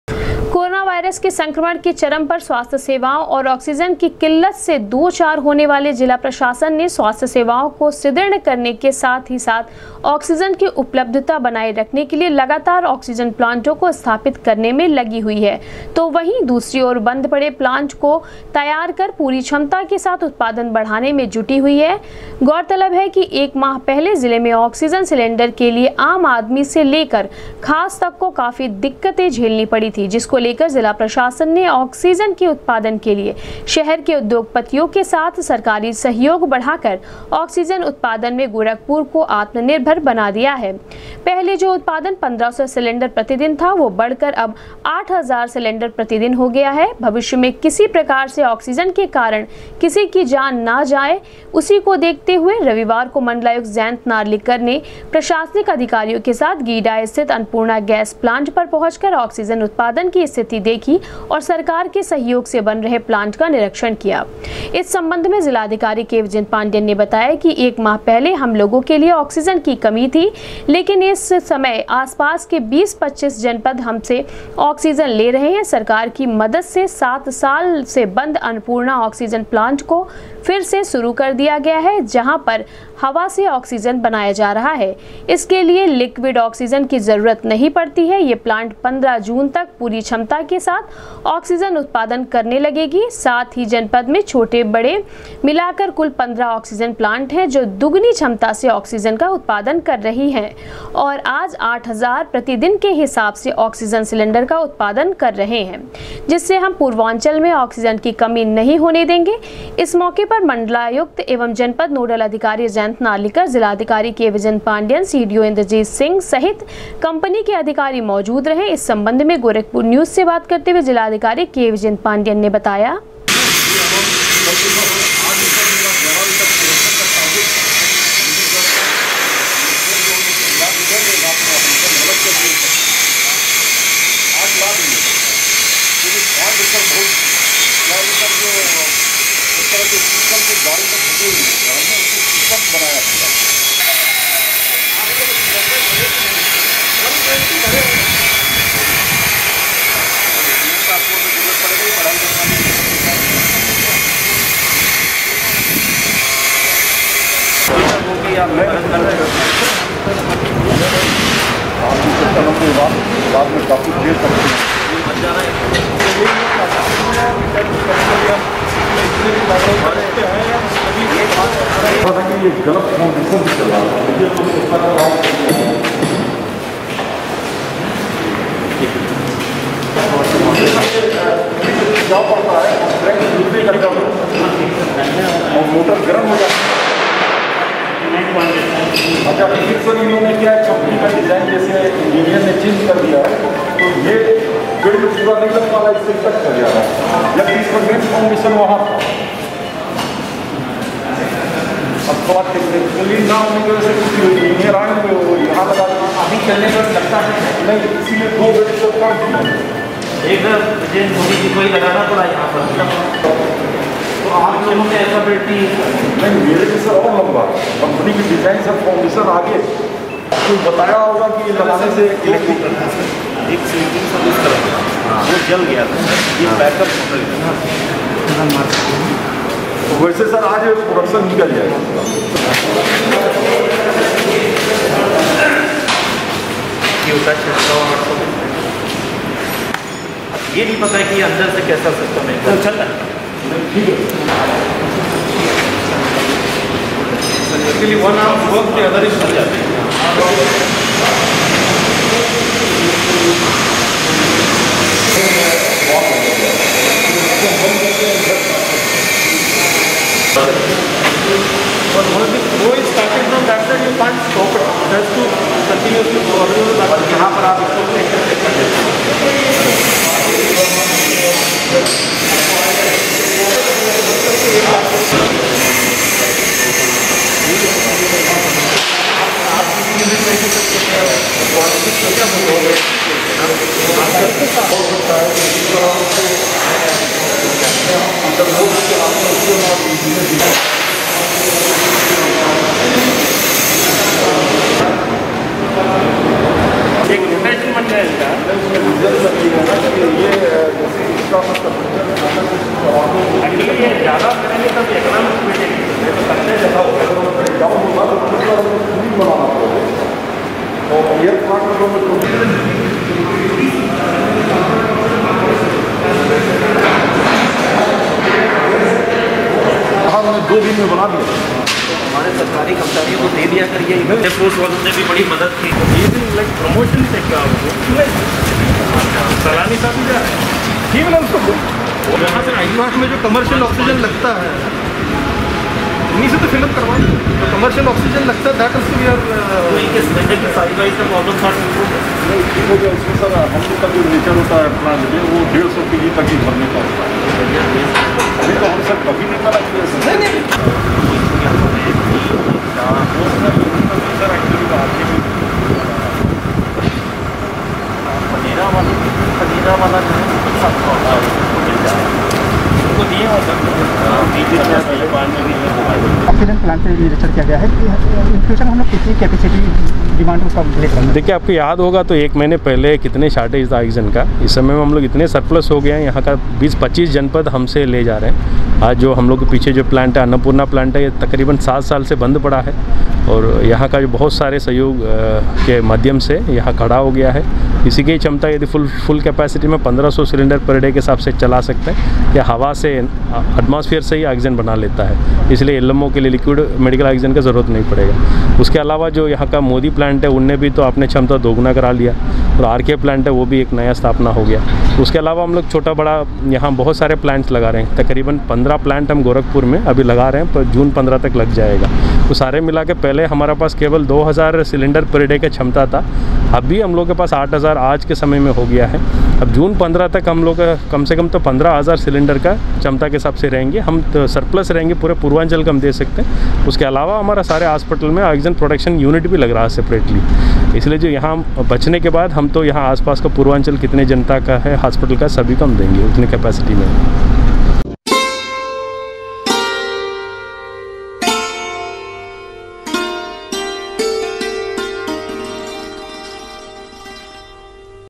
गीडा स्थित अन्नपूर्णा गैस प्लांट का मण्डलायुक्त, डीएम सहित प्रशासनिक अधिकारियों ने किया निरीक्षण
गीडा स्थित अन्नपूर्णा गैस प्लांट का मण्डलायुक्त, डीएम सहित प्रशासनिक अधिकारियों ने किया निरीक्षण, जिला अधिकारी ने कहा, अब पूर्वांचल में आक्सीजन की कमी से नहीं जाएगी किसी की जान